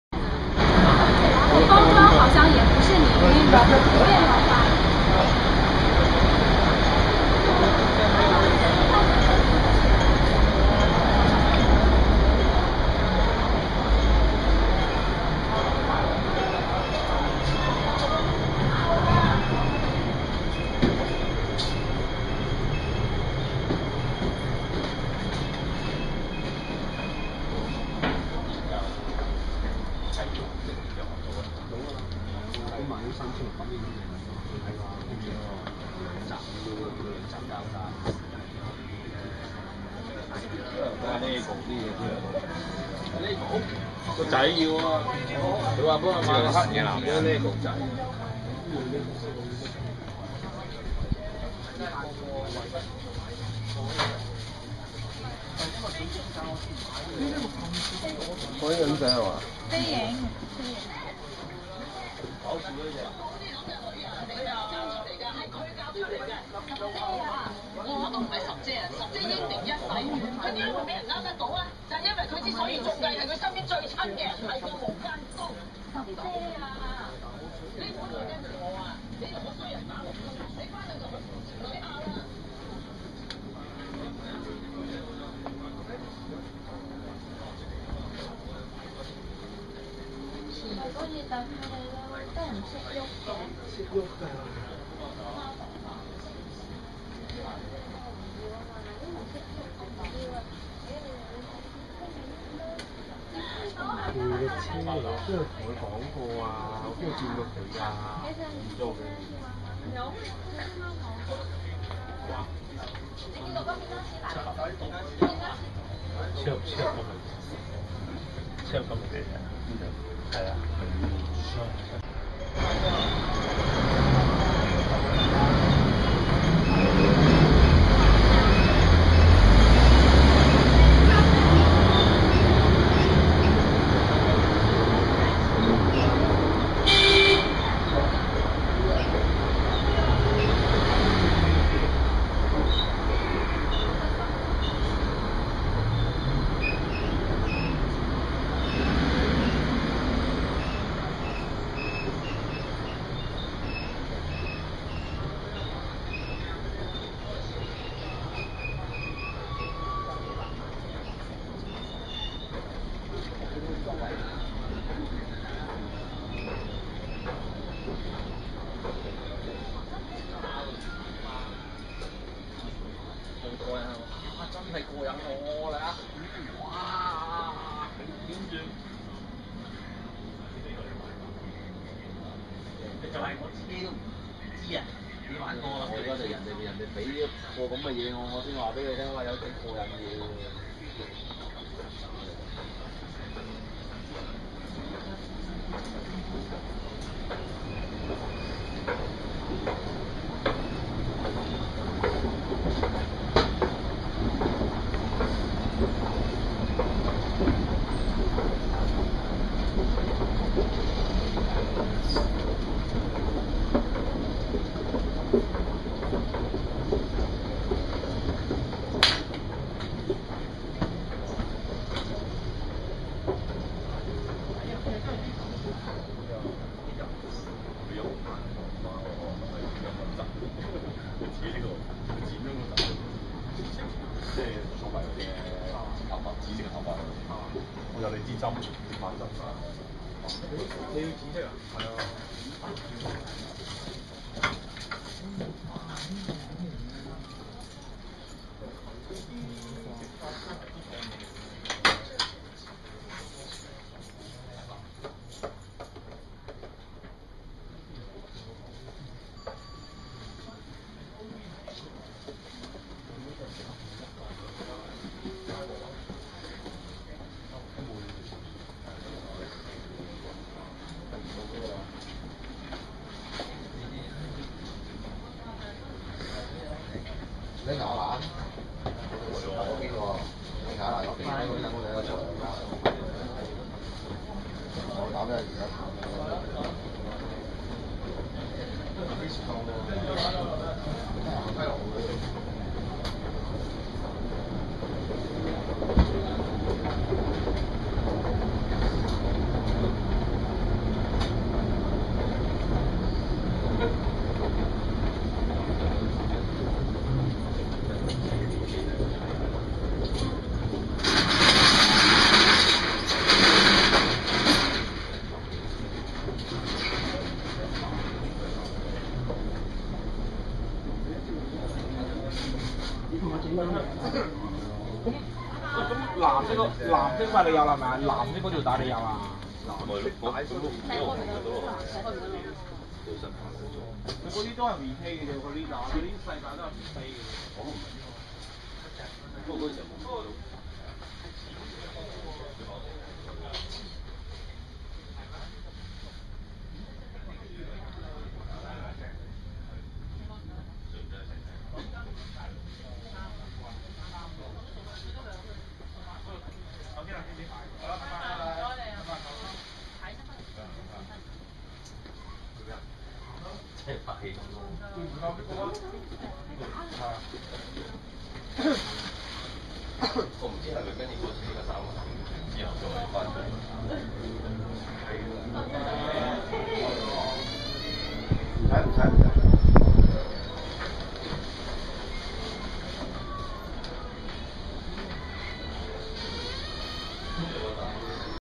Walkthrough Richmond Shopping Arcade 皆旺商場
Follow us as we walkthrough this small basement and upstairs shopping centre dedicated to collectable toy like action figures, scale models and designer toys.

Address. 107-111 Argyle Street 亞皆老街. Mong Kok, Hong Kong